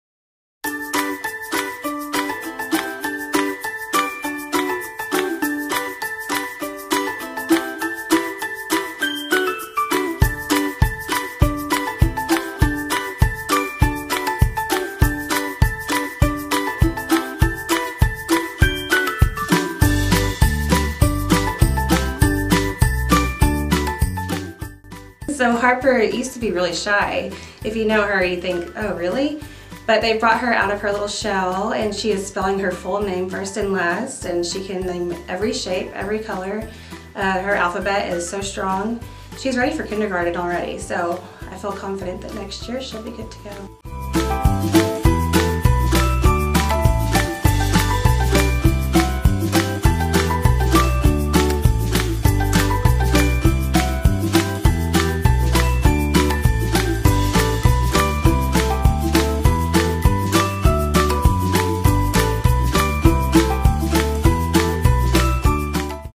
PreK Registration - Chelsea Brents - Video 1
Whitehouse ISD PreK registration will be open April 15th, 2019 - June 13, 2019. More information can be found on the Whitehouse ISD website.